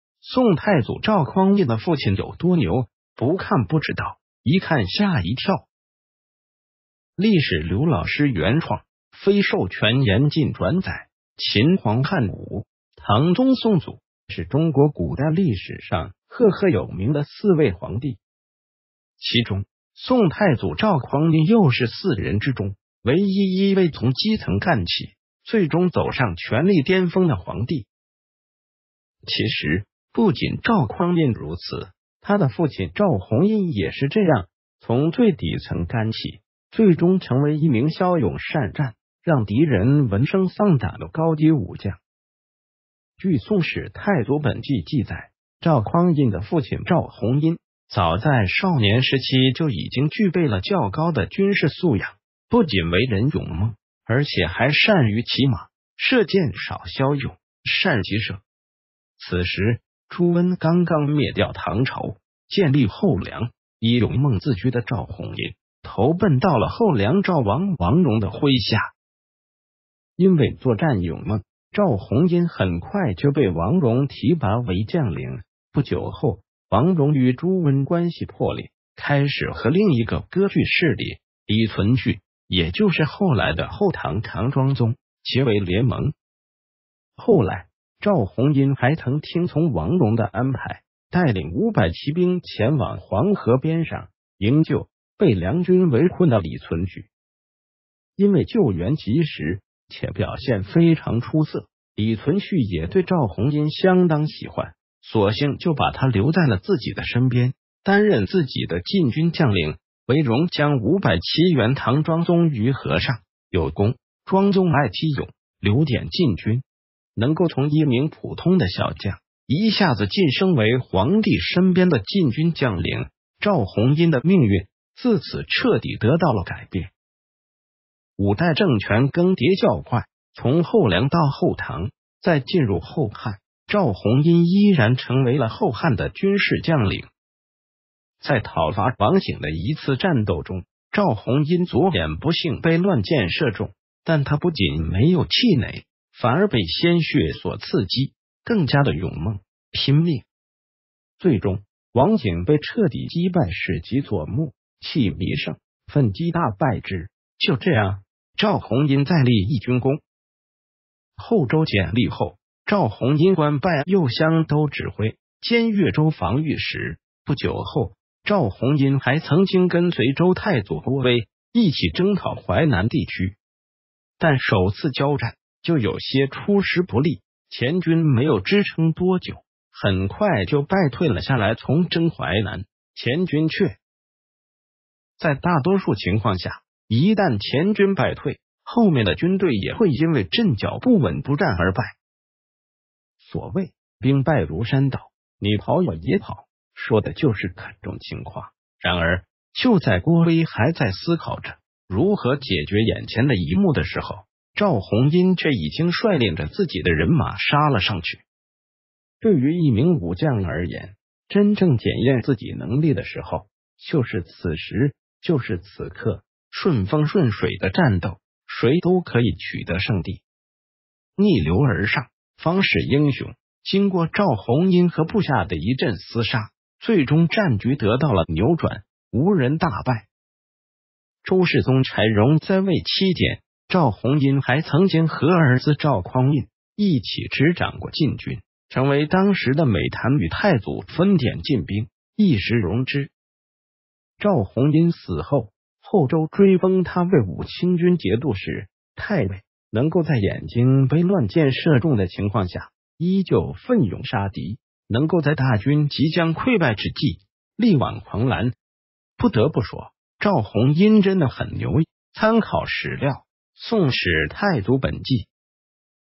宋太祖赵匡胤的父亲有多牛：不看不知道，一看吓一跳
【历史刘老师原创，非授权严禁转载】

“秦皇汉武，唐宗宋祖”，是中国古代历史上赫赫有名的四位皇帝。
其中，宋太祖赵匡胤又是四人之中，唯一一位从基层干起，最终走上权力巅峰的皇帝。
其实，不仅赵匡胤如此，他的父亲赵弘殷也是这样——从最底层干起、最终成为一名骁勇善战、让敌人闻声丧胆的高级武将。
据《宋史·太祖本纪》记载，赵匡胤的父亲赵弘殷，早在少年时期就已经具备了较高的军事素养——不仅为人勇猛，而且还善于骑马、射箭（少骁勇，善骑射）。
此时，朱温刚刚灭掉唐朝，建立后梁。
以勇猛自居的赵弘殷，投奔到了后梁赵王王镕的麾下。

因为作战勇猛，赵弘殷很快就被王镕提拔为将领。
不久后，王镕与朱温关系破裂，开始和另一个割据势力——李存勖（也就是后来的后唐唐庄宗），结为联盟。
后来，赵弘殷还曾听从王镕的安排，带领五百骑兵前往黄河边上营救被梁军围困的李存勖。
因为救援及时，且表现非常出色，李存勖也对赵弘殷相当喜欢，索性就把他留在了自己的身边，担任自己的禁军将领（为镕将五百骑援唐庄宗于河上，有功。庄宗爱其勇，留典禁军）。
能够从一名普通的小将，一下子晋升为皇帝身边的禁军将领，赵弘殷的命运自此彻底得到了改变。
五代政权更迭较快，从后梁到后唐，再进入后汉，赵弘殷依然成为了后汉的军事将领。

在讨伐王景的一次战斗中，赵弘殷左眼不幸被乱箭射中。
但他不仅没有气馁，反而被鲜血所刺激，更加的勇猛、拼命。
最终，王景被彻底击败（矢集左目，气弥盛，奋击大败之）。
就这样，赵弘殷再立一军功。
后周建立后，赵弘殷官拜右厢都指挥，兼岳州防御使。
不久后，赵弘殷还曾经跟随周太祖郭威一起征讨淮南地区。
但首次交战，就有些出师不利——前军没有支撑多久，很快就败退了下来（从征淮南，前军却）。

在大多数情况下，一旦前军败退，后面的军队也会因为阵脚不稳不战而败。
所谓：兵败如山倒，你跑我也跑，说的就是这种情况。
然而，就在郭威还在思考着如何解决眼前的一幕的时候，赵弘殷却已经率领着自己的人马杀了上去。
对于一名武将而言，真正检验自己能力的时候，就是此时，就是此刻！
顺风顺水的战斗，谁都可以取得胜利；
逆流而上，方是英雄！
经过赵弘殷和部下的一阵厮杀，最终战局得到了扭转——吴人大败！
周世宗柴荣在位期间，赵弘殷还曾经和儿子赵匡胤一起执掌过禁军，成为当时的美谈（与太祖分典禁兵，一时荣之）。
赵弘殷死后，后周追封他为武清军节度使、太尉。
能够在眼睛被乱箭射中的情况下，依旧奋勇杀敌；能够在大军即将溃败之际，力挽狂澜。
不得不说，赵弘殷真的很牛！
参考史料：《宋史·太祖本纪》